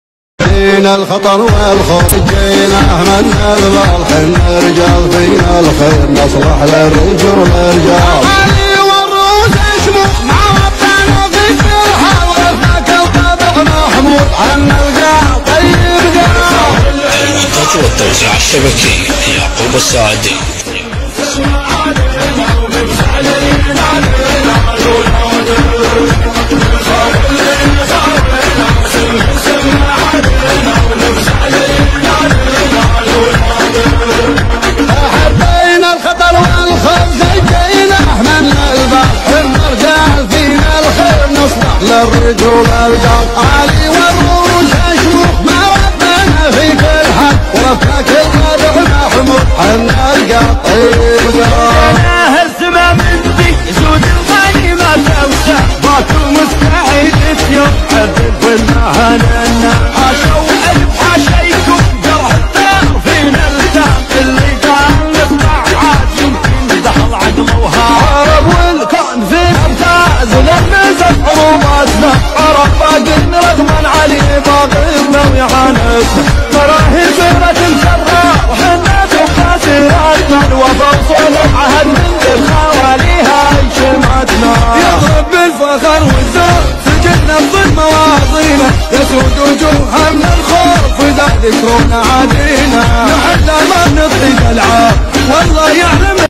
حضور امير قبيله بني اسد على جريمه الشعله المغدورين الاخوه الثلاثه التي هزت الشعله
جريمه الشعله التي هزت منطقه الشعله على يد قتله جبناء كما ادى الامر الى احضار اقوى شيوخ بني اسد وما زالت بني اسد تتواجد في منطقه الشعله وكل يوم تخرج وتدخل شيوخ التي منعت من دخول اي زرفي من الدخول الى منظقه الشعله